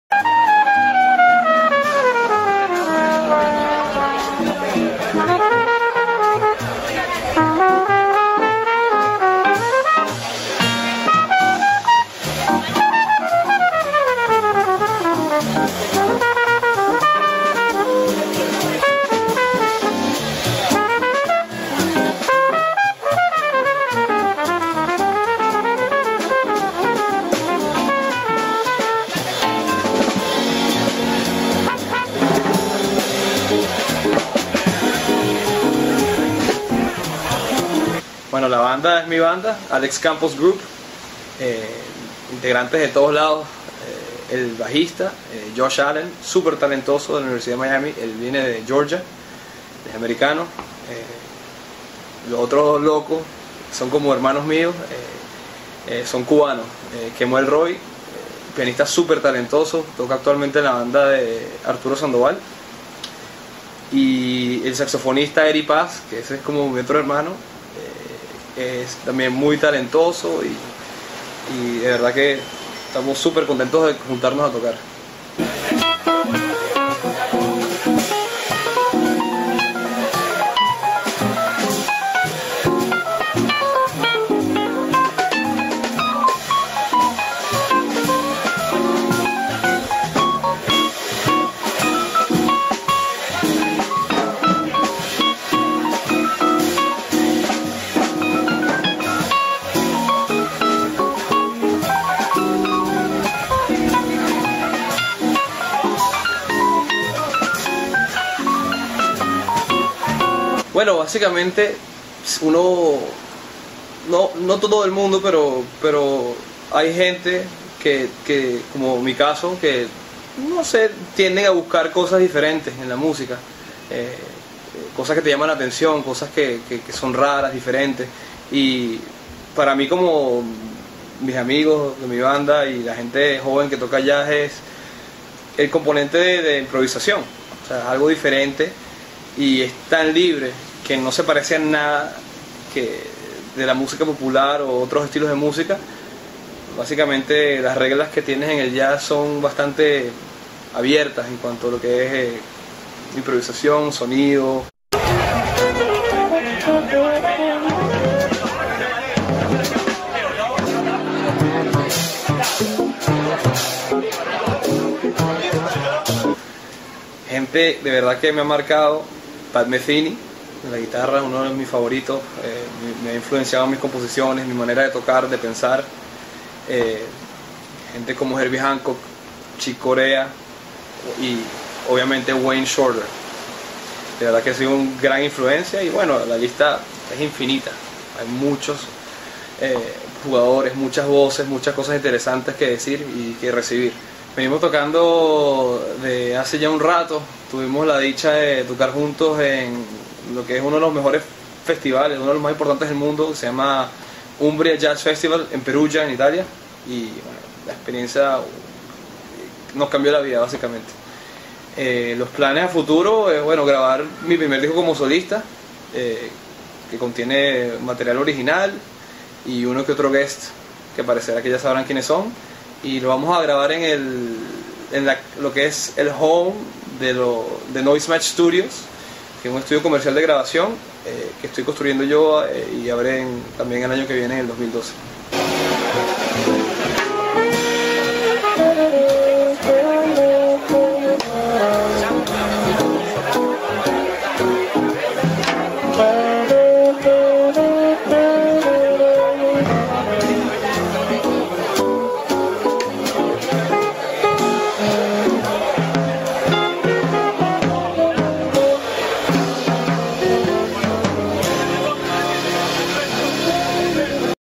Jazz in Miami - Alex Campos
- La banda de Alex Campos. Jovenes que apuestan al jazz en Miami.